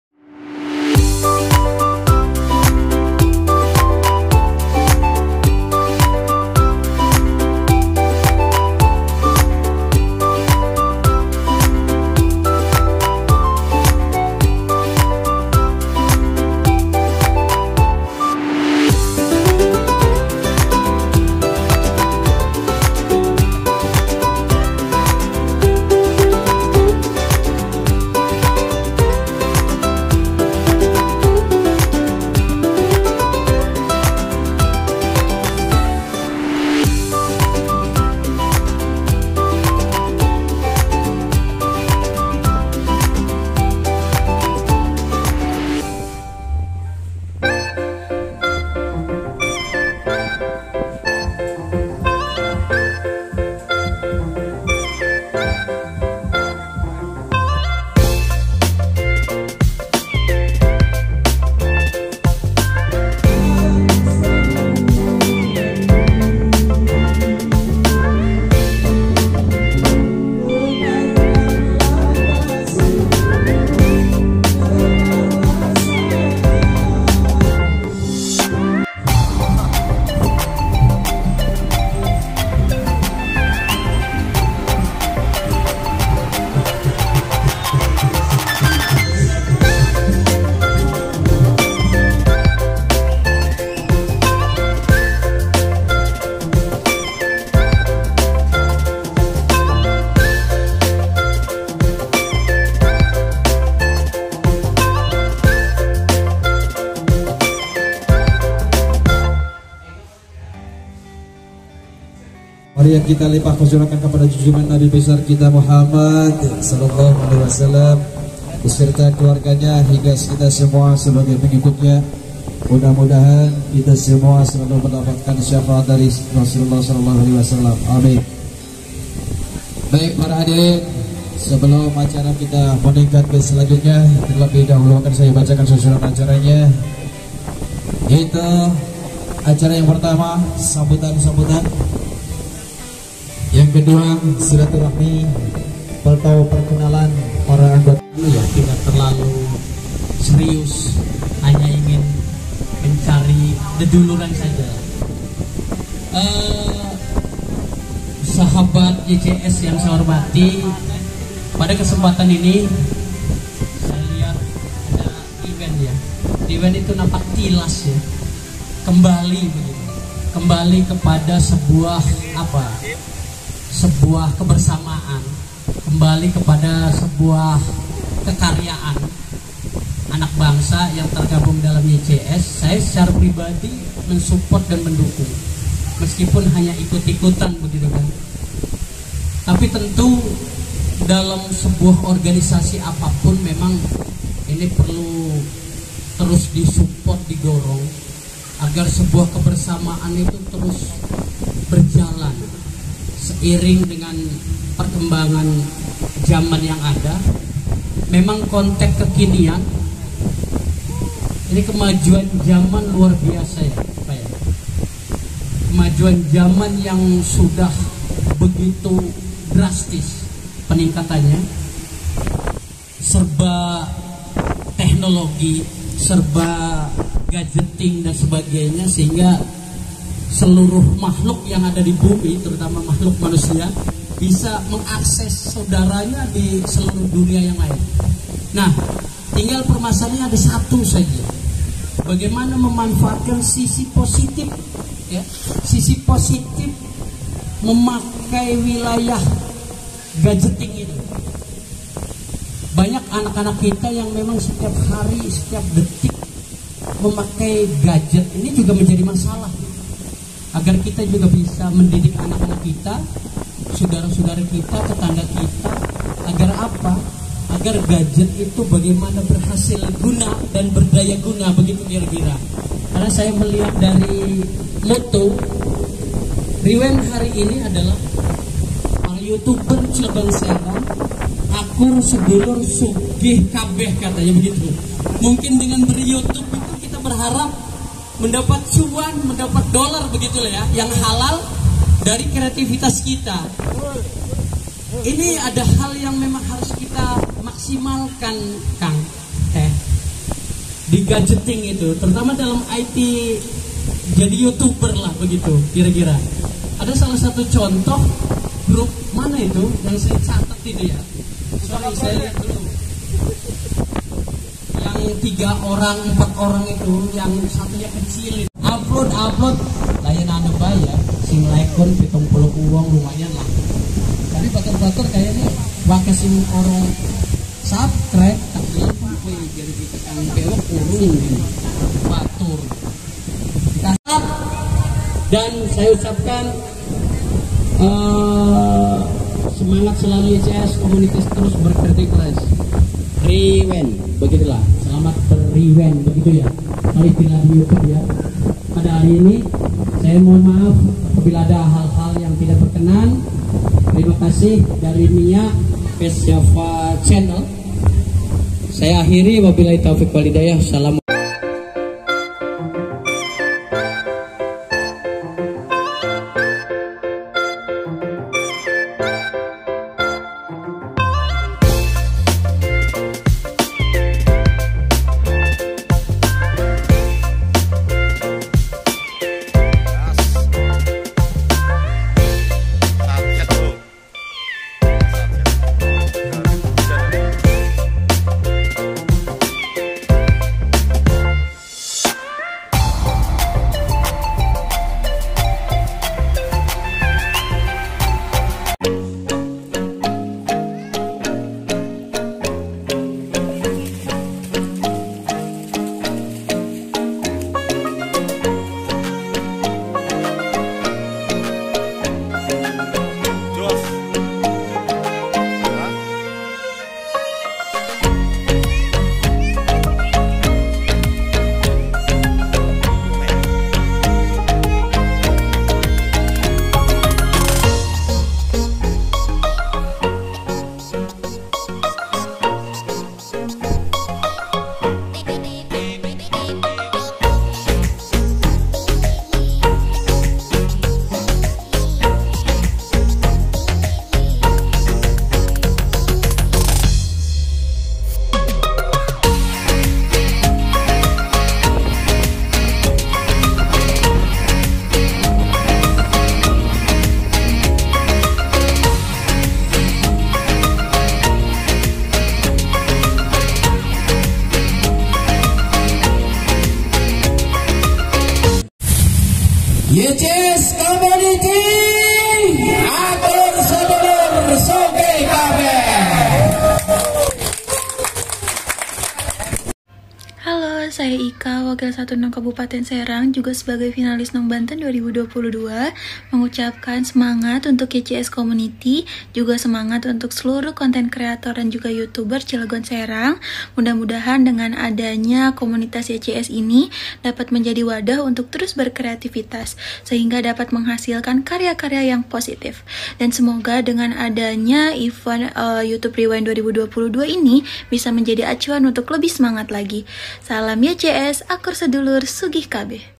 VILLA ROKAN ANYER Acara YouTube Rewind 2022 Bersama YCS Community
Video ini dibuat hanya untuk Dokumentasi saja, Semoga Acara ini Tetap berlanjut dan semakin sukses Jaya Amin...

YCS adalah Singkatan Dari YouTuber Cilegon Serang Yang pertama Kali dibentuk Oleh:
Ketua: Bpk Hendra (ADC Banten Channel)
Wakil ketua: Kang ewok (Hayfa Studio Channel)

Dan Anggota Yang terdiri Sekitar Puluhan peserta Lebih...

Yg ikut youtube rewind 2022 :
1. Hendra - ADC Banten
2. Ahong - Ahong Sepotong
3. Sasha - ADC Banten
4. Rohim - ADC Banten
5. Rohman - ADC Banten
6. Agfy - ADC Banten
7. Ismat - ADC Banten
8. Amel - ADC Banten
9. Suhada - Suka Suka Channel
10. Mang Bah Channel
11. Riva - Suka Suka Channel
12. Tsagif - ADC Banten
13. Roy - Roy Tutorial Channel
14.Tanto - Mahapatih official
15. Ridho - Kertar Serdang
16.sueb - DETEKTIVE MISTERY
17. Ridwan - Eva&Ridwan
18. Jay - Tengah Wengi
19. Eenk Dul Kenyot - Fito Channel
20. Agus - Fito Channel
21. Ika - ADC Banten
22. Ichan Piperales
23.Kang Ewok Hayfa
24.Farhan creator
25.Dharma Pusakaprojct
26.Genta Pusaka Project
27.jhoni Pusaka Project
28.Alan Pusaka Project
29.toet Pusaka Project
30.jenal Pusaka Project
 Studio
31. Irul / Alfa Car Audio
32. Aden kasep official
33. Jawara Halal Indonesia
34. Mengetuk Pintu Langit
35. The Indonesian Culture
36. Bang aY Jayalaksana
37. Wilip Online
38. Sarbini Halim
39 egga indran - egga indran gumelar
40. Pak Dewan Ibrohim - MIA West Java
41. Gobang Channel
Dan lainnya Yang belum kesebut silahkan Komentar...

Lanjutkan yg blm ..